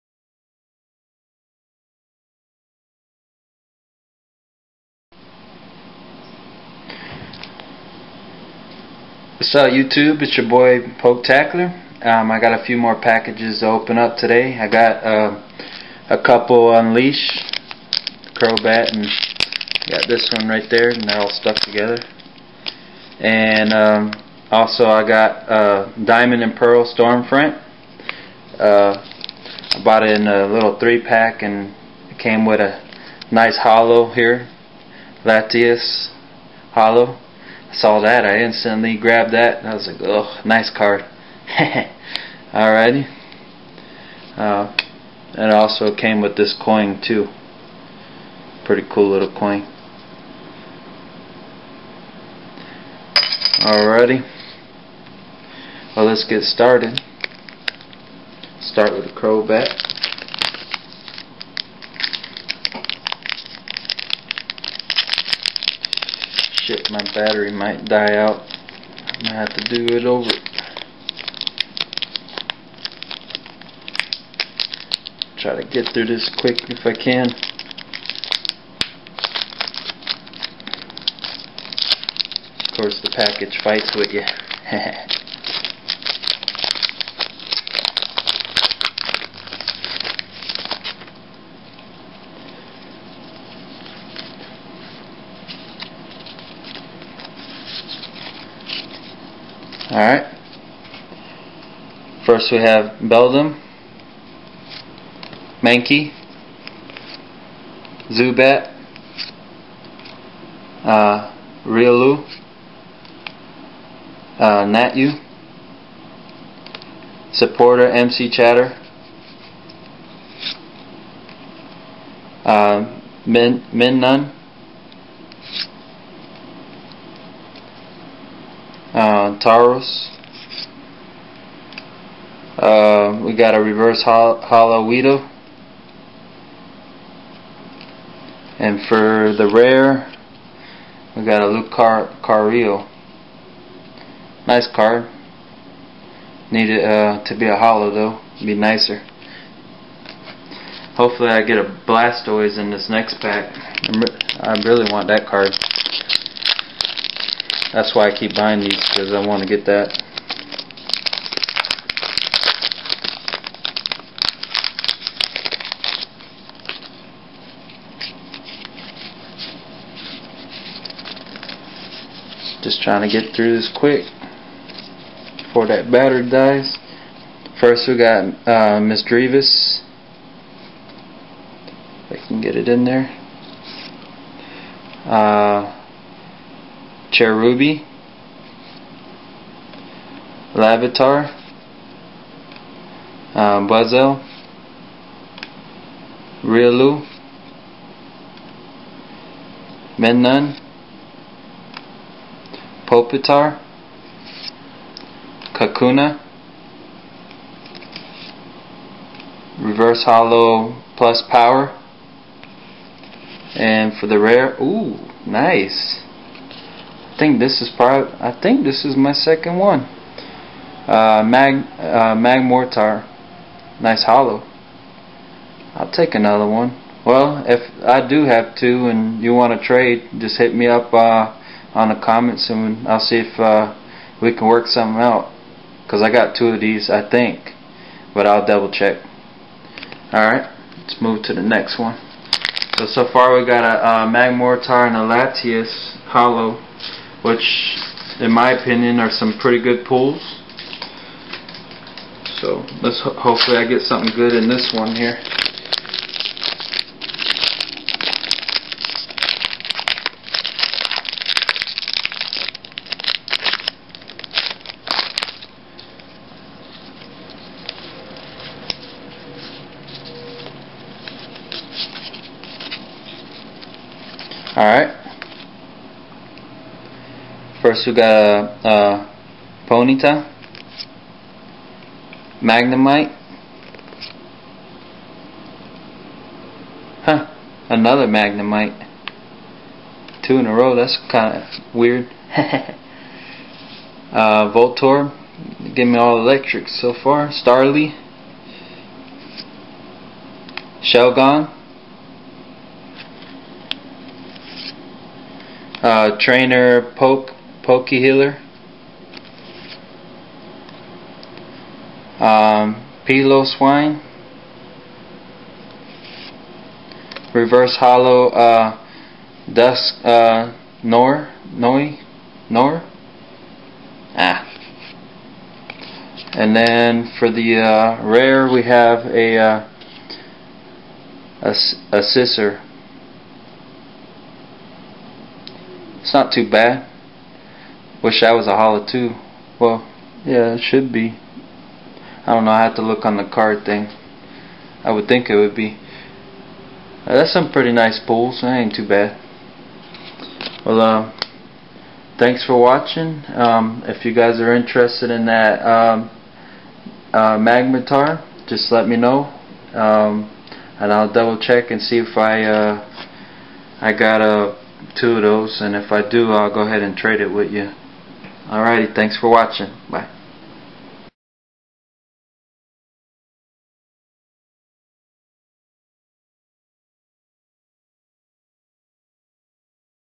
Opening Unleashed Pokemon Booster Packs (Good Pulls)
Opening a few booster pack from a three pack. 2 Unleashed and one Diamond and Pearl Stom Front Series. Also, I learn how to work my camera, so the picture quality is alittle better than before. You can now see the letters on the cards. YAY! I said in the video that if i have 2 of those Magmortars I would trade one of them. Well I have 2, but one is reverse holo and the other one I just got was a regular holo. So, I don't if i want to trade it, but i guess it depends on what you  wanna offer.:)